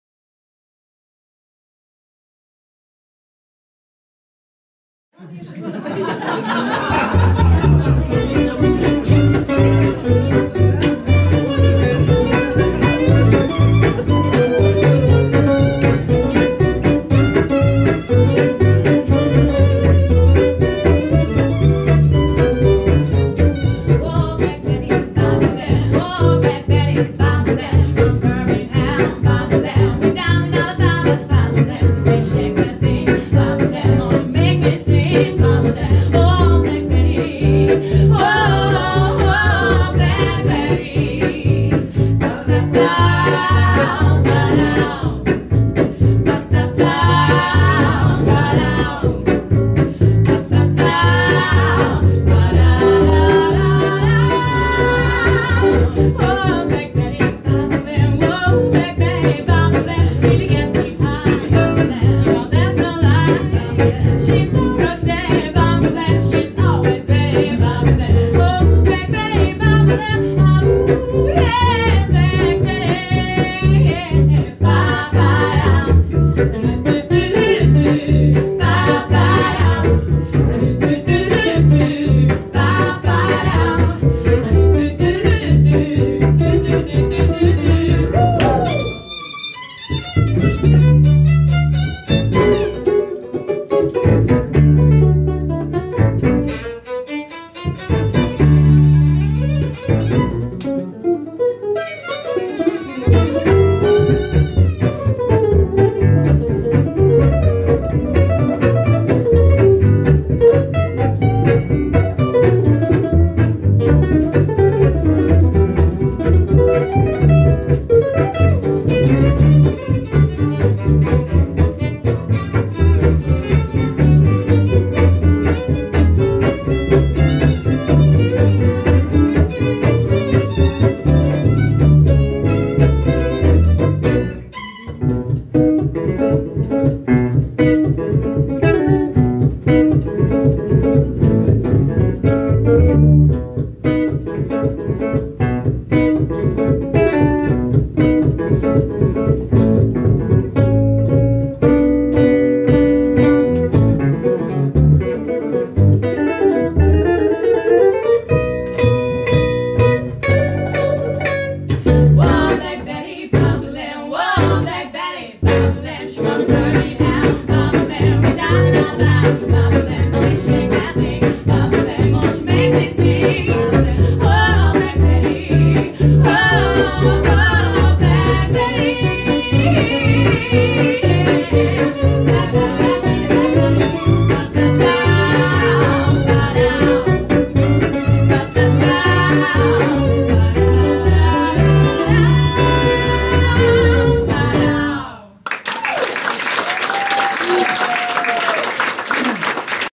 Urban Gipsy & Anna-Maria part 01
Новая программа, которую Urban Gipsy подготовили для Вас совместно с дуэтом Анна-Мария.
Состав:
Анна и Мария Опанасюк - вокал
Дмитрий Кушниров - гитара
Дмитрий Коваленко - гитара
Матвей Миронов - скрипка
Николай Кистенев - контрабас